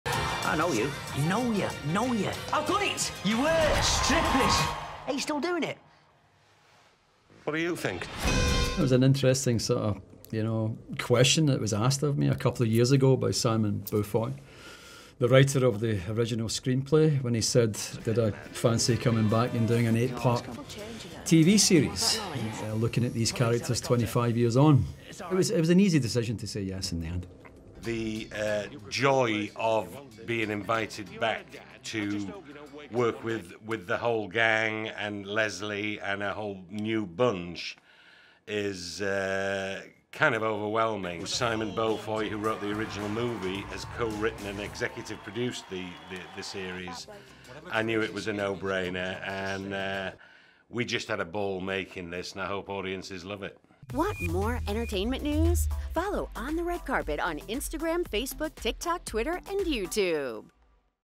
"The Full Monty" cast interview - 25 years later!
25 years later and the gang from "The Full Monty" is back in action. TheFullMonty, the series, is now streaming on @hulu